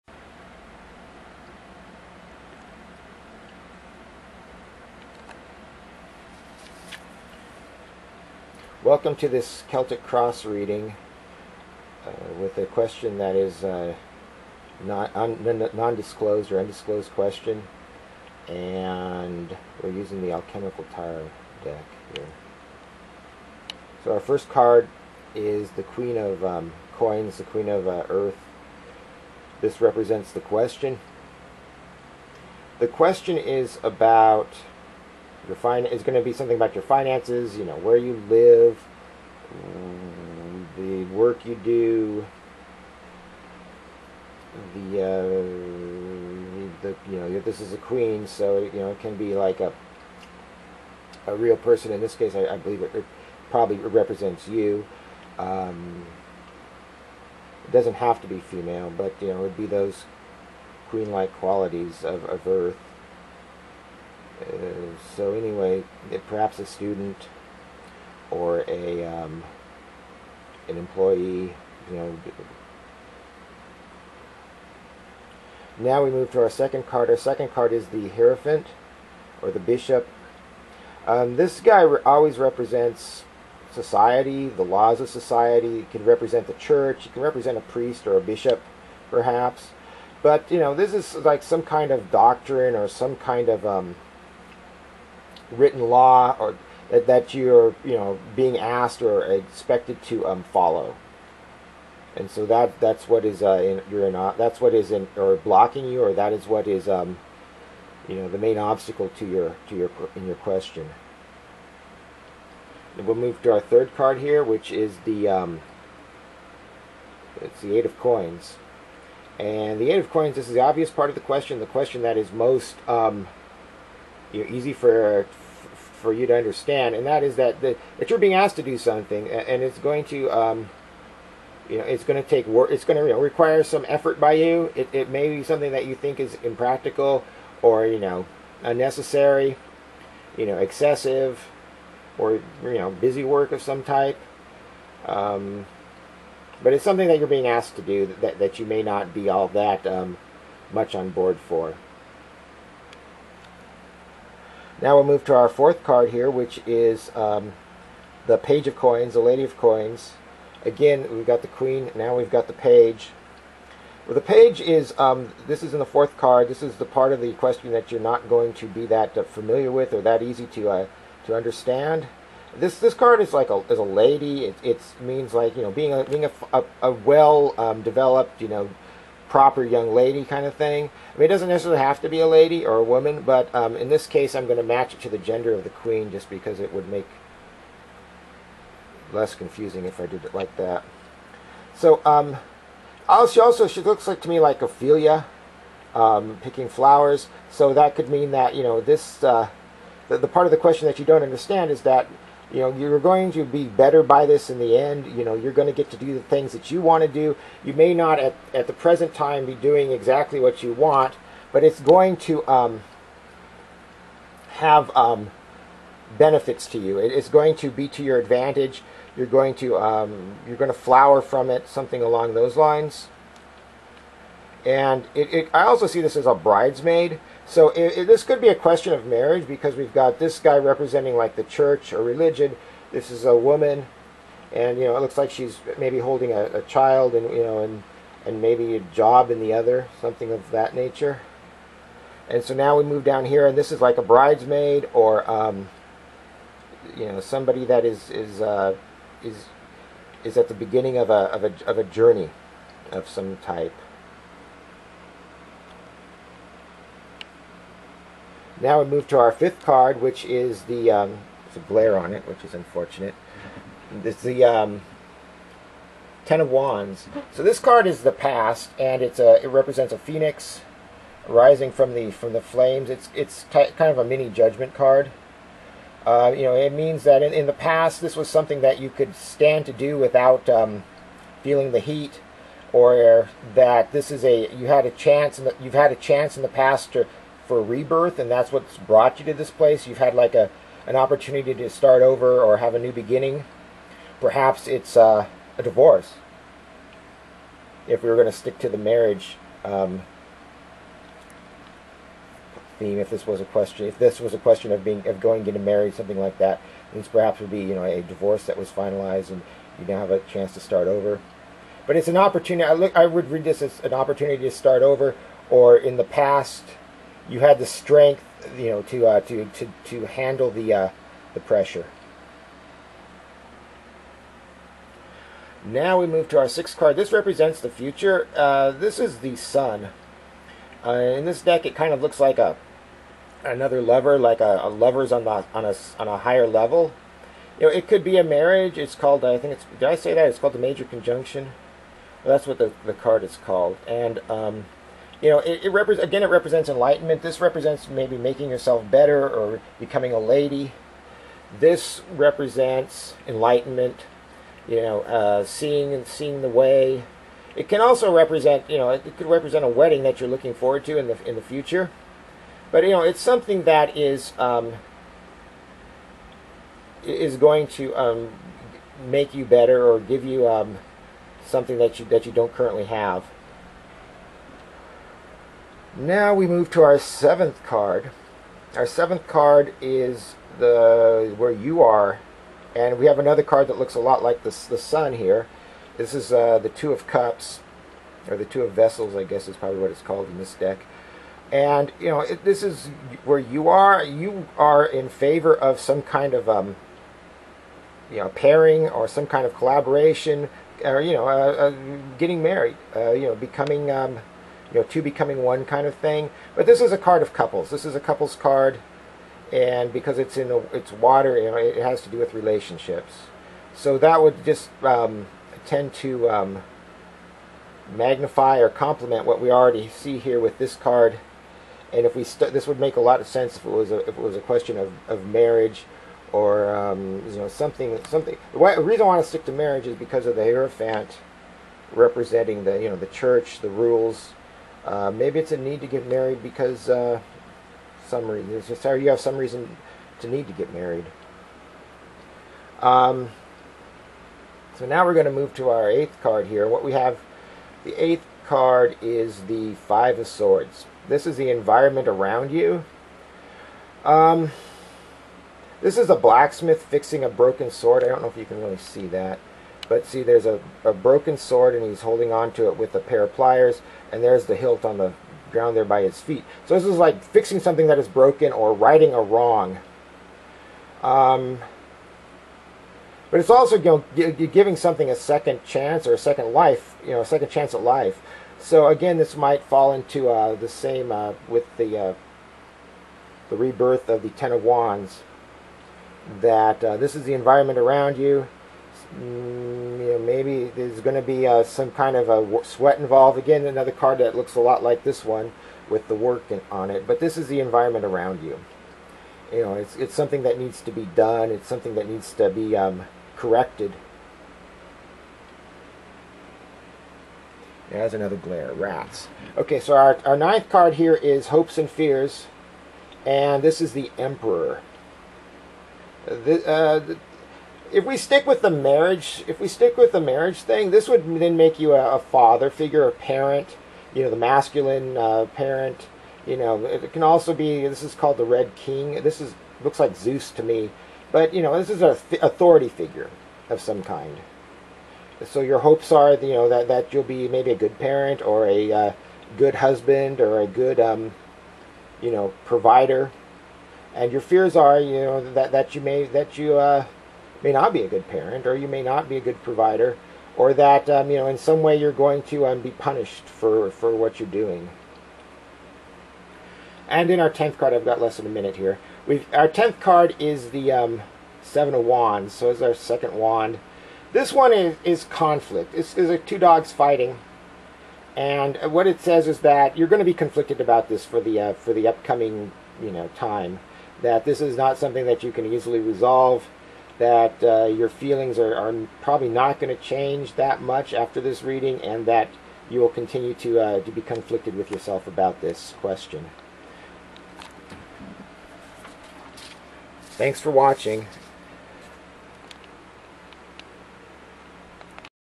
Celtic Cross Reading using Alchemical Tarot for the first time.
The question was a secret. This is my first reading using the Alchemical Tarot. It starts with the Queen of Coins and ends with of Wands.

The video is somewhat shaky and the sound fades in and out.  I apologize for not being able to edit the video currently.

I am warning you in advance to be careful if you are using ear-buds not to turn up the volume too much at the start of the video. It starts with no sound and gets loud fast.